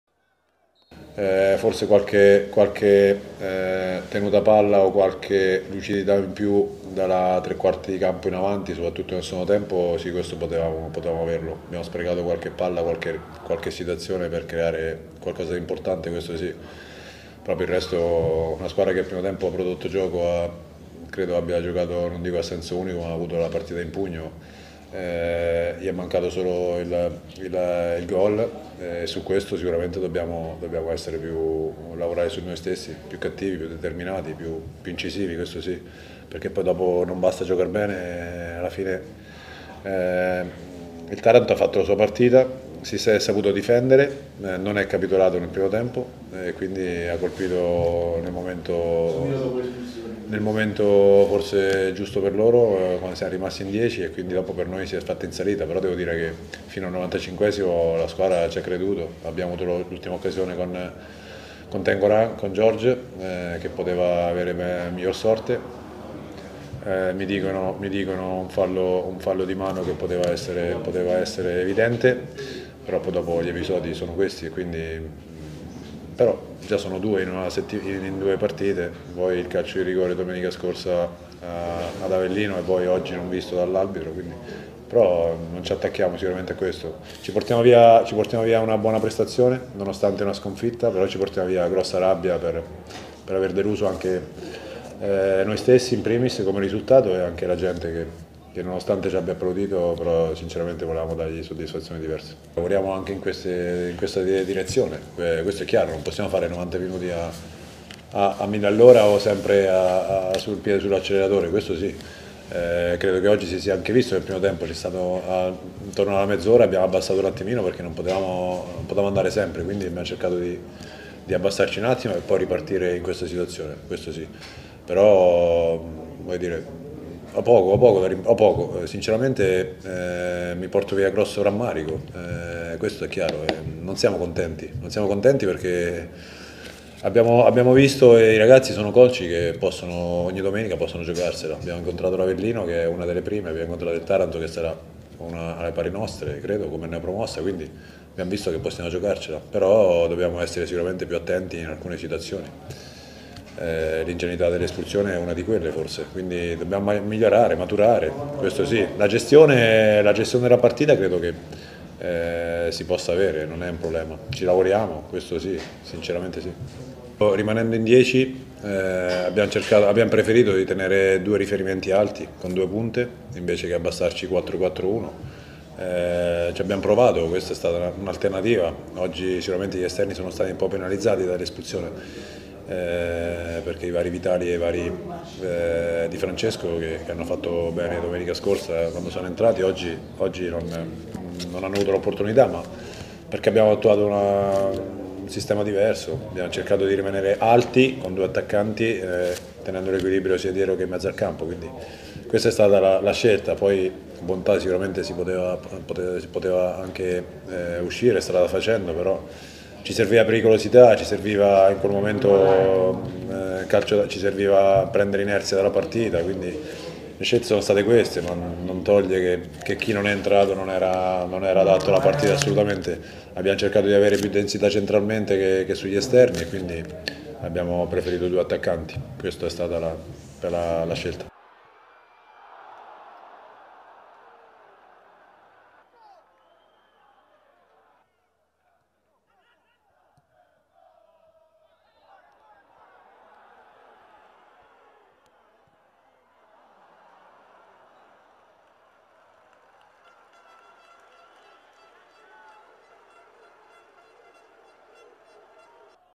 Campobasso, Cudini commenta lo stop col Taranto: è mancato solo il gol - 06/09/...
Servizi TG | Campobasso, Cudini commenta lo stop col Taranto: è mancato solo il gol | 6/9/2021